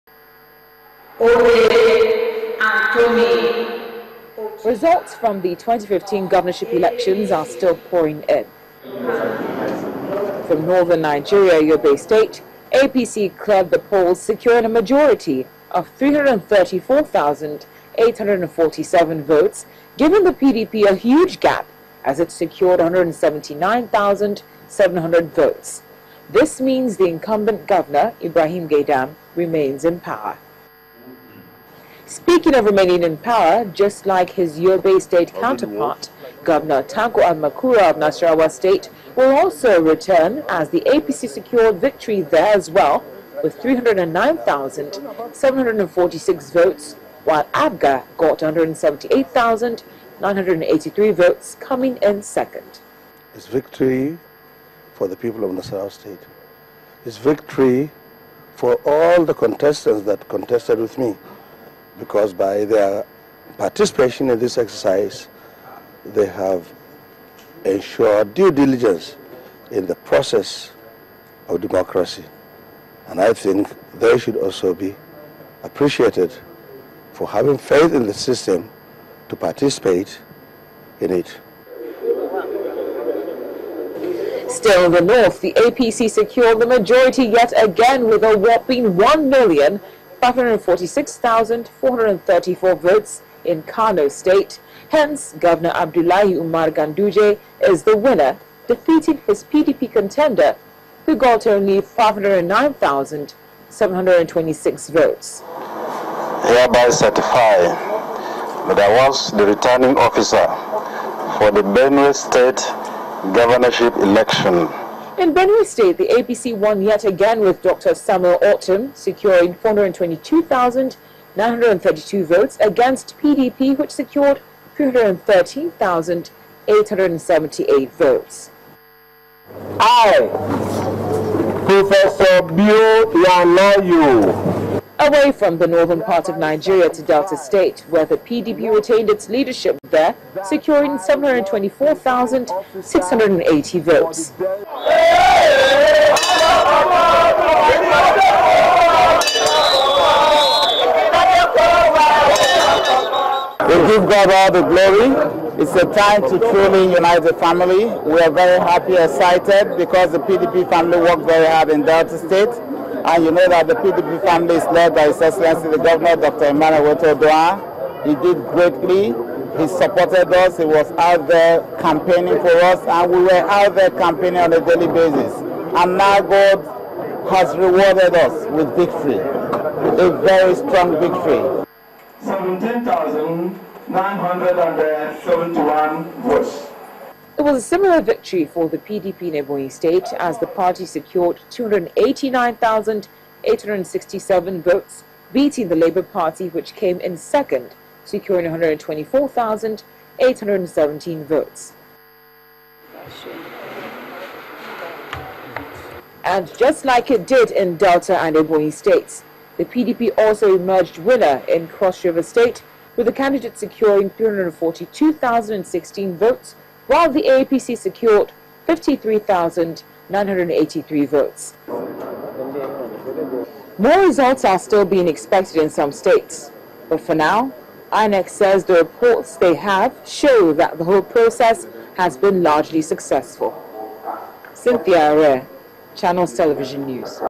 Governorship Election Results Across Nigeria 140415
The results of the governorship election held in Nigeria, as announced by the Independent National Electoral Commission on Monday.